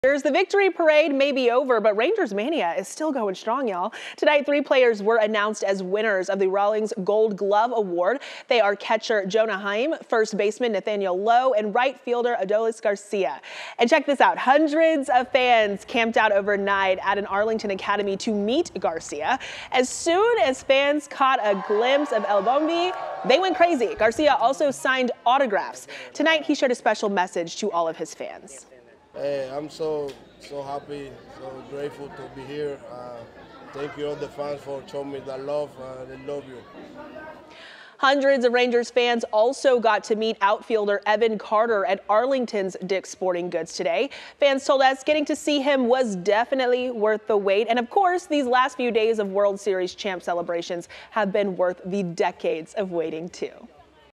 Three Texas Rangers selected for 2023 Rawlings Gold Glove Awards
Jonah Heim, Nathaniel Lowe and Adolis Garcia were announced as winners at their respective positions in the American League.